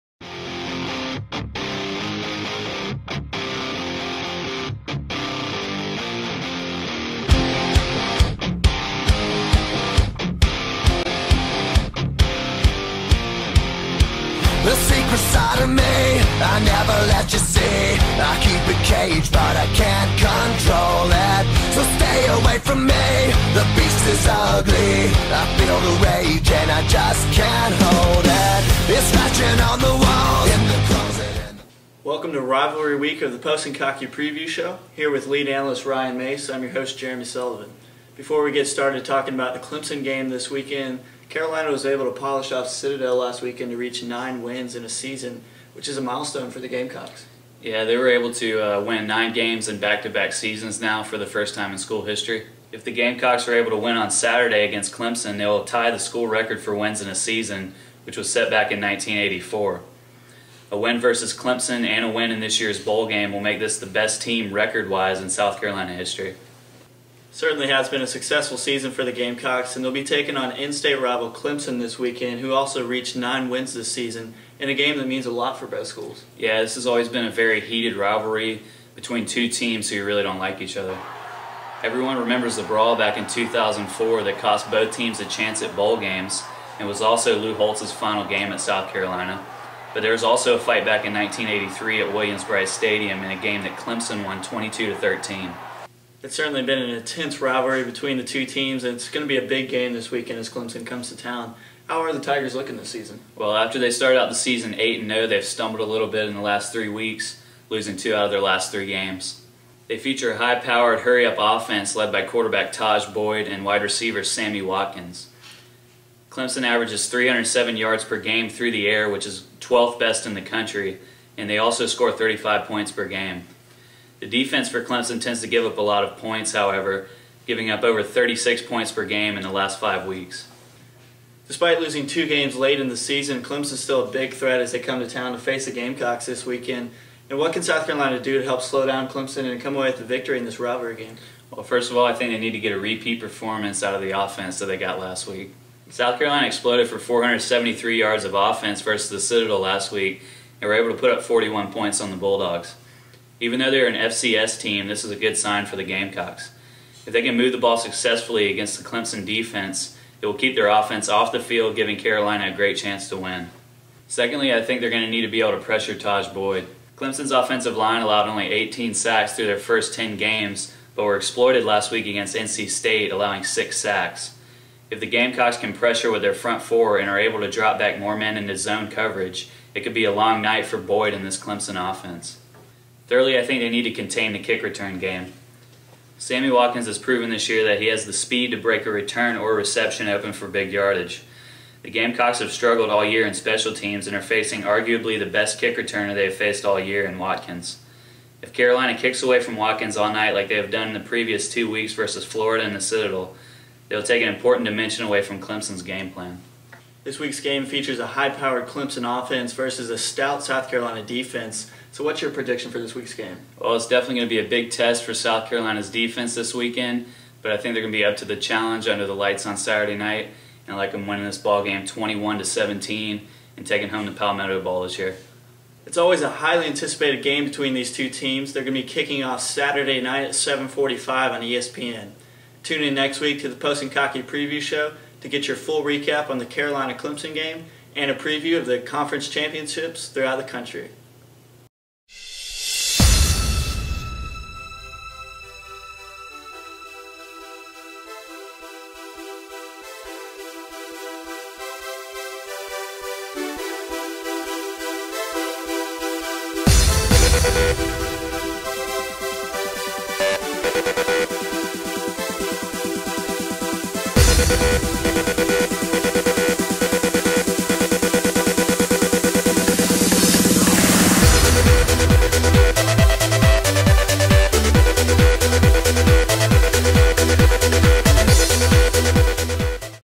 South Carolina - Clemson
The Post and Cocky Preview Show of the South Carolina Clemson game.